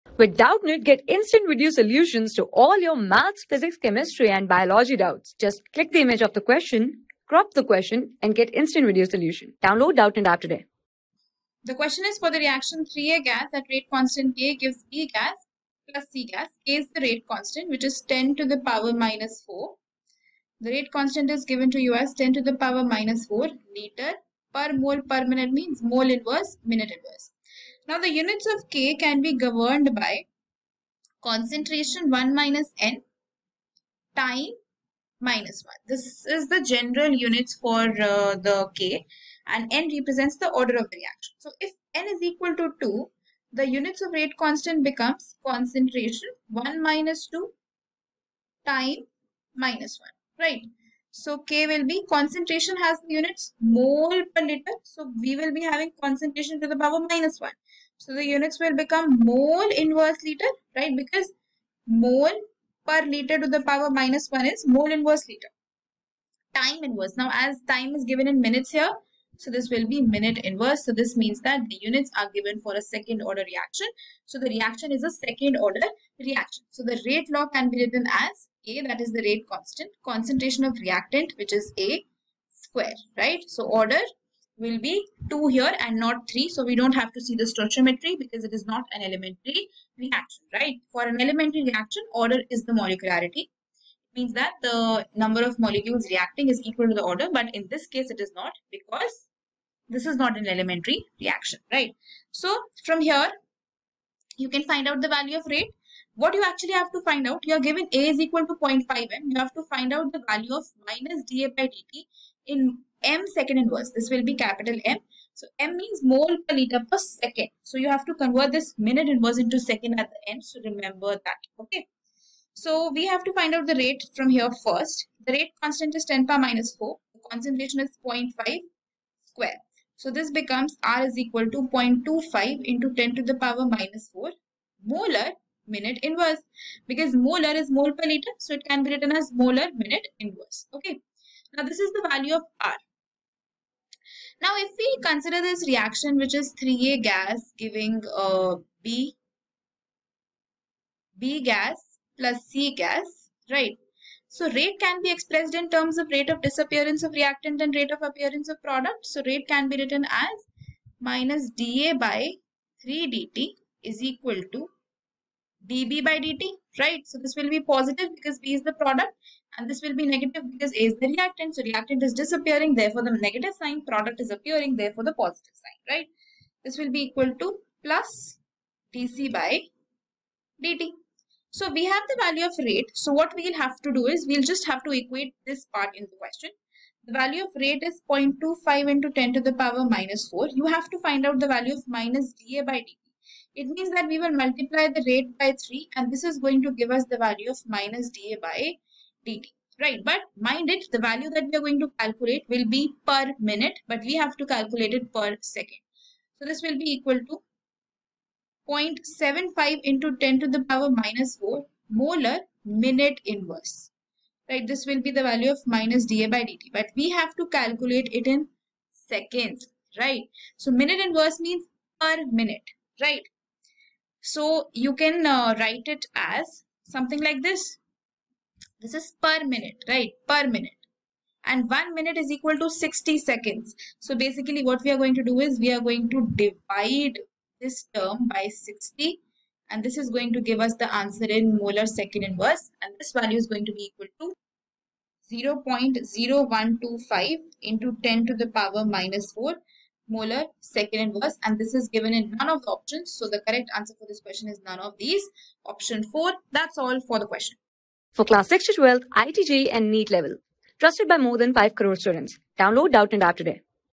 For the reaction 3A(g)overset(k)toB(g)+C(g) k is 10^(-4)L//mol.min. If [A] = 0.5M then the value...
For the reaction 3A(g)overset(k)toB(g)+C(g) k is 10^(-4)L//mol.min. If [A] = 0.5M then the value of -(d[A])/(dt)(in ms^(-1)is:
Class: 12
Subject: CHEMISTRY
Chapter: CHEMICAL KINETIC & NUCLEAR CHEMISTRY
Board:IIT JEE

👉 Have Any Query? Ask Us.